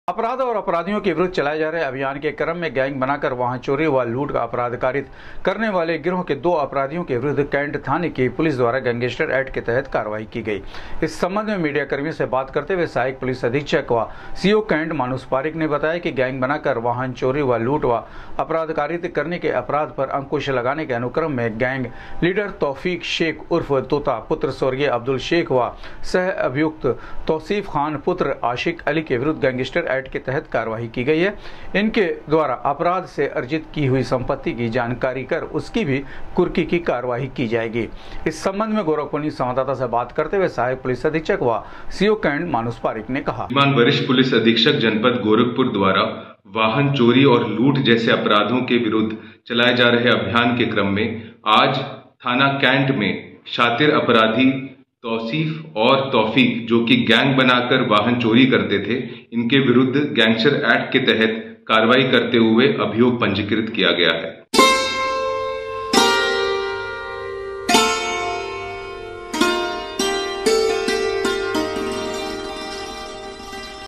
गैंग बनाकर वाहन चोरी व लूट का अपराध कारित करने वाले गिरोह के 2 अपराधियों के विरुद्ध
गैंग बनाकर वाहन चोरी व लूट का अपराध कारित करने वाले गिरोह के 2 अपराधियों के विरुद्ध कैण्ट थाने की पुलिस द्वारा गैंगस्टर एक्ट के तहत की गई करवाई, सहायक पुलिस अधीक्षक व सीओ कैण्ट मानुस पारीक ने दी जानकारी